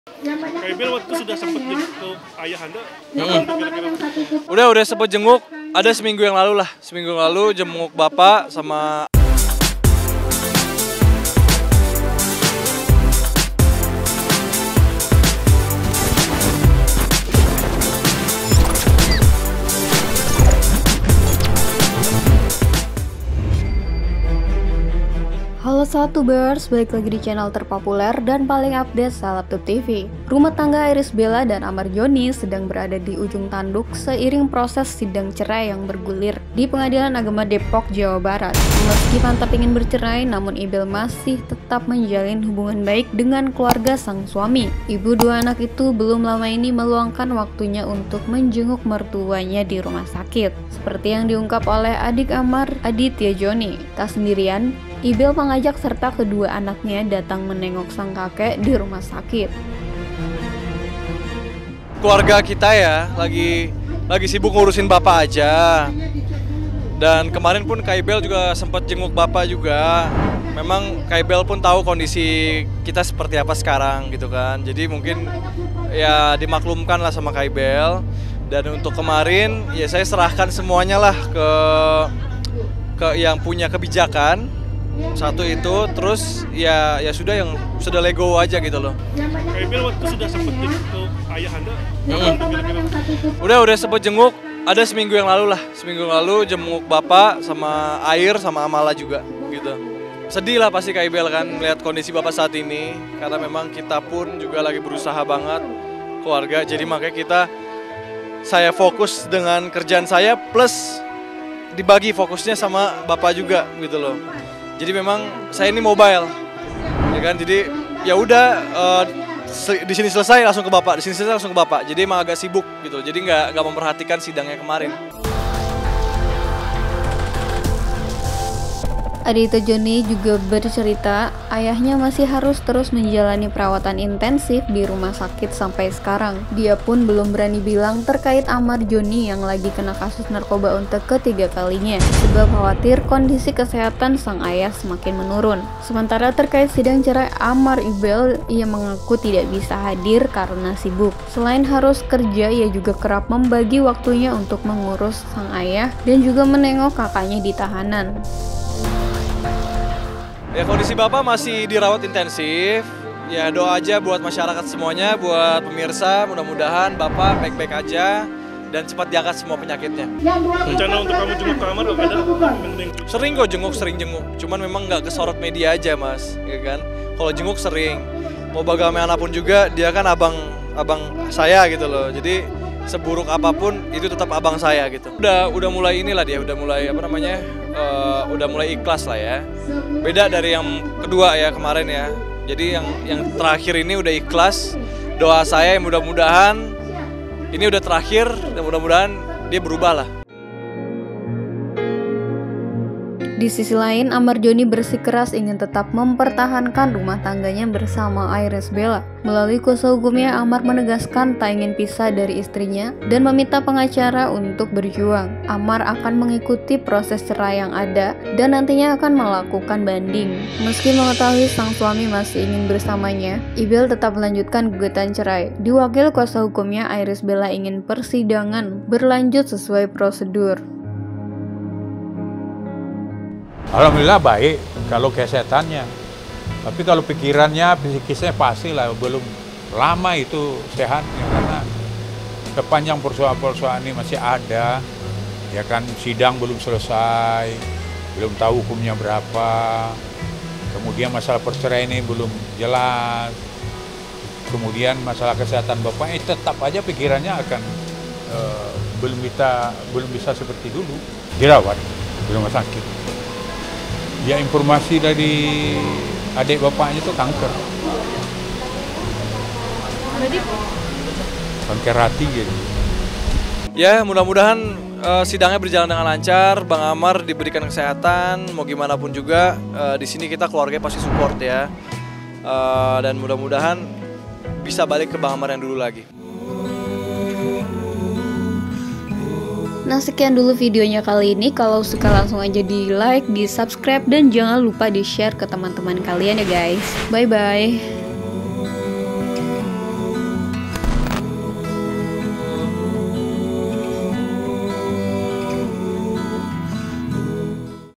IRISH BELA JENGUK AYAH MERTUA, SINYAL MASIH PEDULI AMMAR ZONI?
Hello SelebTuber. !!